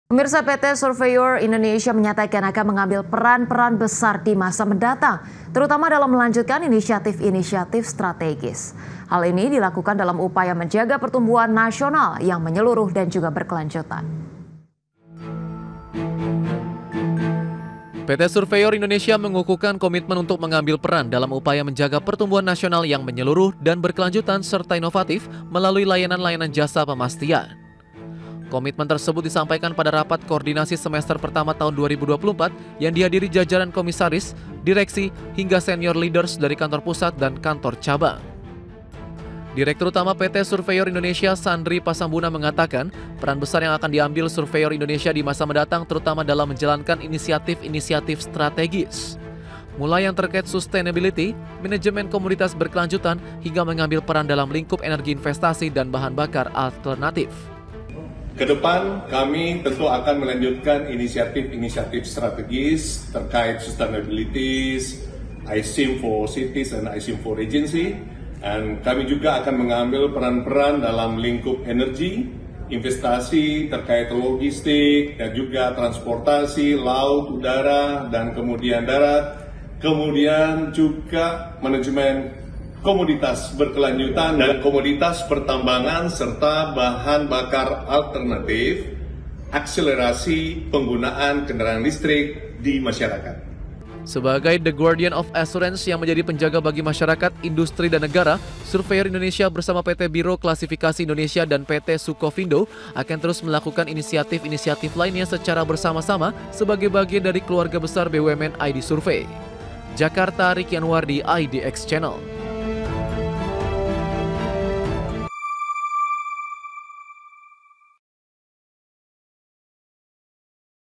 Surveyor Indonesia Perkuat Komitmen Dalam Pembangunan | IDX CHANNEL
PT Surveyor Indonesia menyatakan, akan mengambil peran-peran besar di masa mendatang, terutama dalam melanjutkan inisiatif-inisiatif strategis. Hal ini dilakukan dalam upaya menjaga, pertumbuhan nasional yang menyeluruh dan juga berkelanjutan.